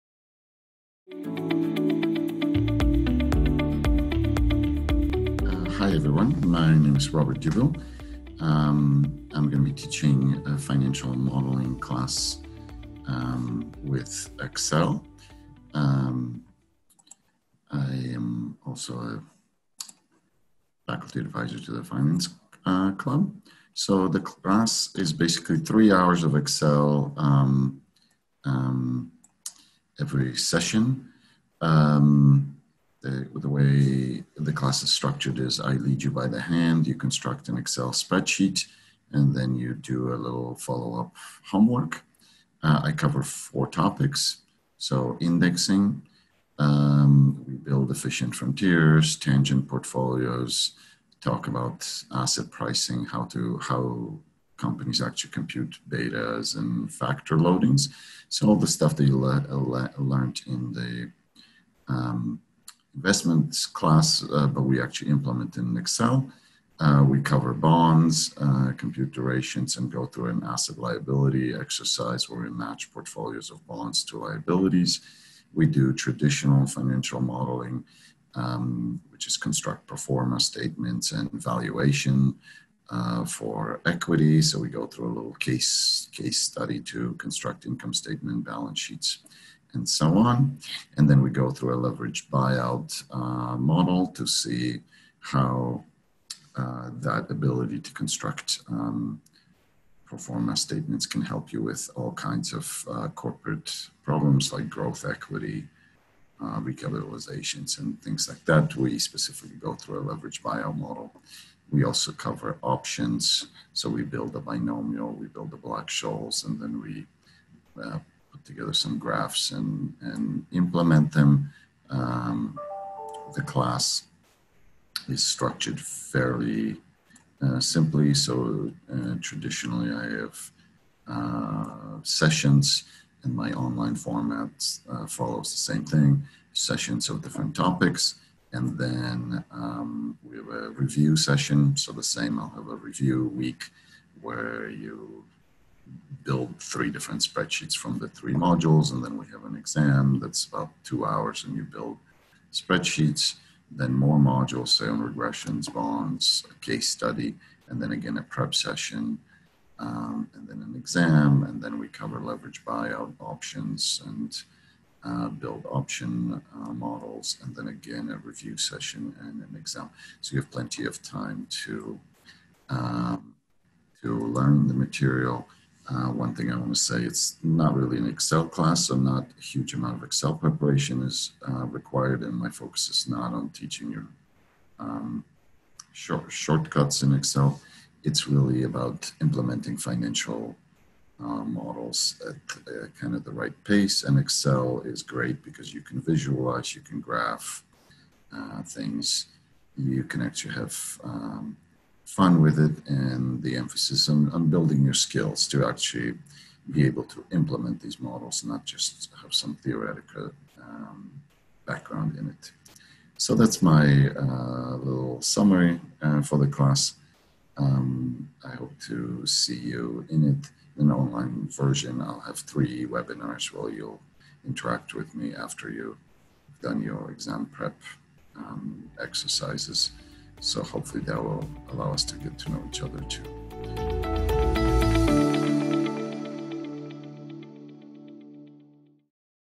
Finance Electives: FIN 5380 with Robert Dubil
David Eccles School of Business Professor (Lecturer) of Finance Robert Dubil describes what students can expect from FIN 5380.